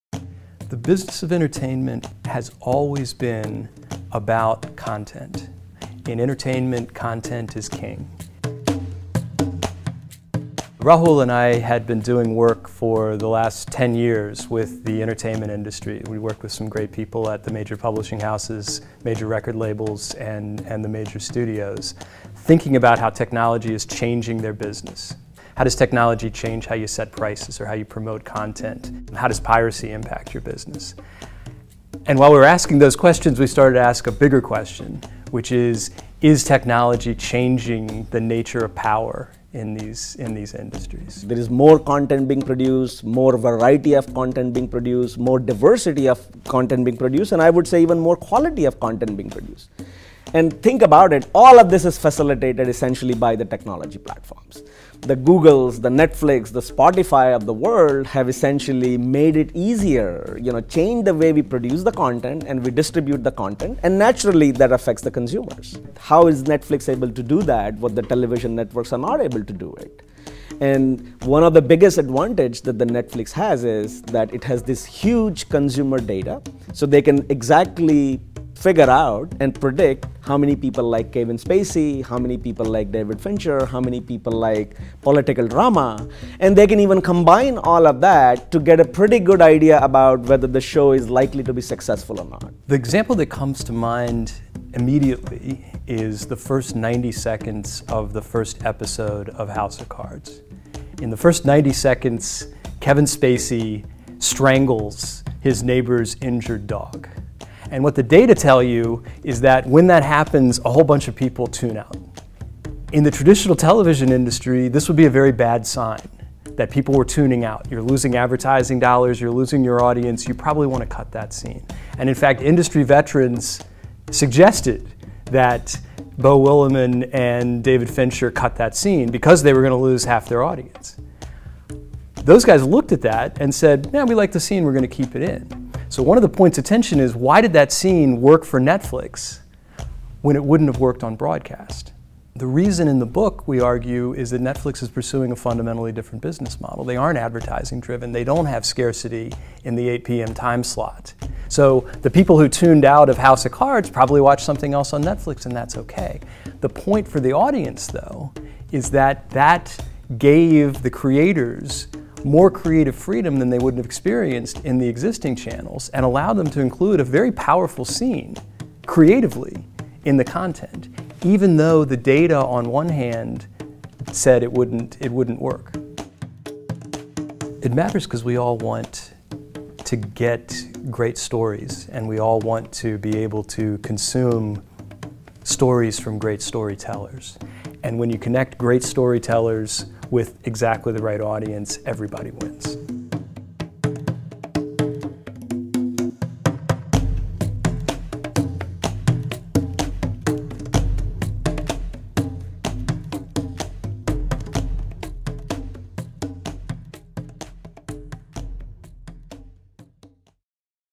"Streaming, Sharing, Stealing: Big Data and the Future of Entertainment"
Heinz College professors Michael Smith and Rahul Telang discuss their new book, "Streaming, Sharing, Stealing: Big Data and the Future of Entertainment."